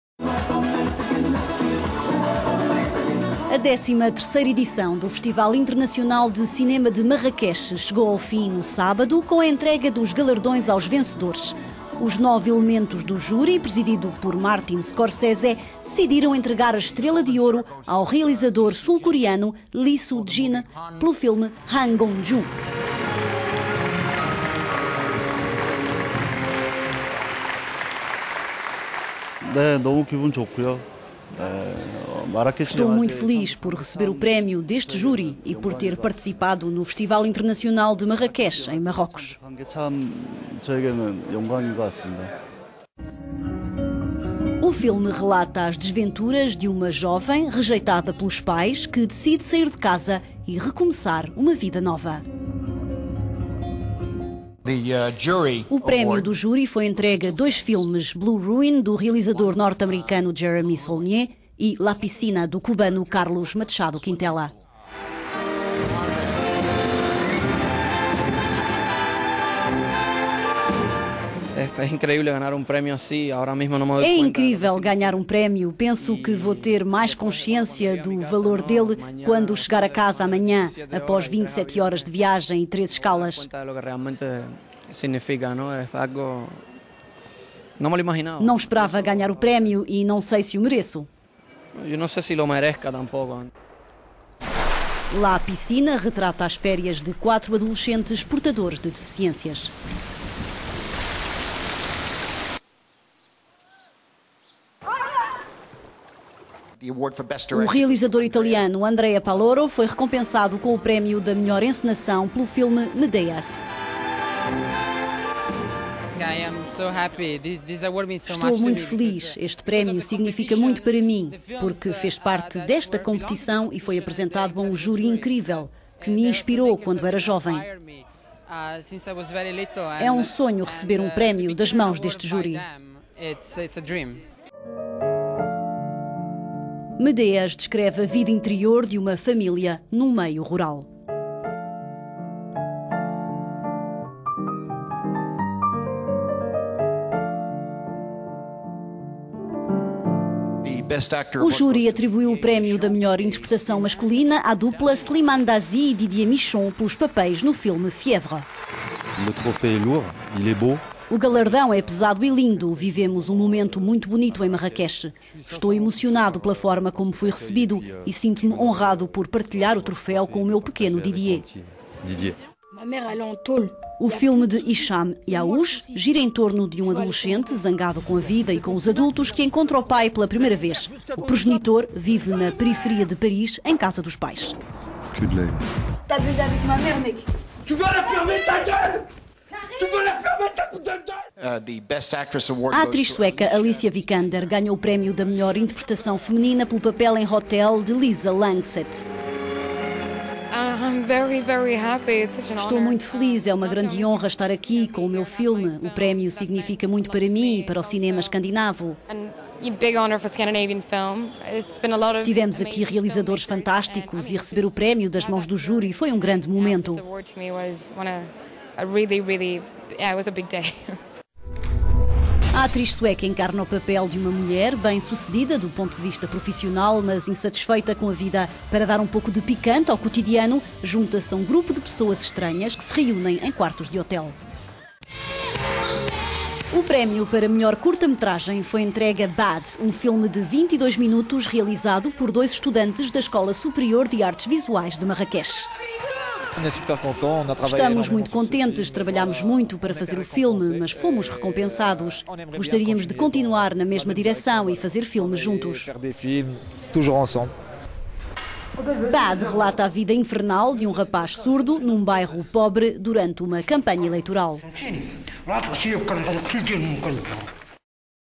Lee Su-Jin vence festival de Cinema de Marraquexe - cinema
O júri do Festival Internacional de Cinema de Marraquexe presidido por Martin Scorsese  entregou a...

O júri do Festival Internacional de Cinema de Marraquexe presidido por Martin Scorsese  entregou a estrela de ouro ao realizador sul-coreano Lee Su-Jin pelo filme "Han Gong-Ju".

"Estou muito feliz por receber o prémio deste júri e por ter participado no Festival Internacional de Marrequexe em Marrocos", disse o realizador sul-coreano.

"Han Gong-Ju" relata as desventuras de uma jovem rejeitada pelos pais que decide sair de casa e recomeçar uma vida nova.

O prémio do júri foi entregue a dois filmes, "Blue Ruin" do realizador norte-americano Jeremy Saulnier e "La piscina" do cubano Carlos Machado Quintela.

"É incrível ganhar um prémio, penso que vou ter mais consciência do valor dele quando chegar a casa amanhã após 27 horas de viagem e três escalas, não esperava ganhar o prémio e não sei se o mereço", confessou o realizador cubano.

"La piscina" retrata as férias de quatro adolescentes portadores de deficiência.

O realizador italiano Andrea Pallaoro foi recompensado com o prémio da melhor encenação pelo filme "Medeas".

"Estou muito feliz, este prémio significa muito para mim, porque fez parte desta competição e foi apresentado a um júri incrível que me inspirou quando era jovem, é um sonho receber um prémio das mãos deste júri", disse o realizador italiano.

"Medeas" descreve a vida interior de uma família, num meio rural.

O júri atribuiu o prémio da melhor interpretação masculina à dupla Slimane Dazi e Didier Michon pelos papéis no filme "Fièvres".

"O galardão é pesado e lindo, vivemos um momento muito bonito em Marraquexe, estou emocionado pela forma como fui recebido e sinto-me honrado por partilhar o troféu com o meu pequeno Didier", disse o ator francês.

O filme de de Hicham Ayouch gira em torno de um adolescente zangado com a vida e com os adultos que encontra o pai pela primeira vez. O progenitor vive na periferia de Paris em casa dos pais.

A atriz sueca Alicia Vikander ganhou o prémio da melhor interpretação feminina pelo papel em "Hotell", de Lisa Langseth.

"Estou muito feliz  é uma grande honra estar aqui com o meu filme, o prémio significa muito para mim e para o cinema escandinavo. Tivemos aqui realizadores fantásticos e receber o prémio das mãos do júri foi um grande momento", considerou Alicia  Vikander.

A atriz sueca incarna o papel de uma mulher, bem sucedida do ponto de vista profissional mas insatisfeita com a vida.
Para dar um pouco de picante ao quotidiano, junta-se a um grupo de pessoas estranhas que se reúnem em quartos de hotel.

O prémio para melhor curta-metragem foi entregue a "Bad", um filme de 22 minutos,realizado por Ayoub Lahnoud e Alaa Akaaboune, dois estudantes da escola superior de artes visuais de Marraquexe.

"Estamos muito contentes. Trabalhámos muito para fazer o filme mas fomos recompensados, gostaríamos de continuar na mesma direção e fazer filmes juntos", disse Alaa Akaaboune.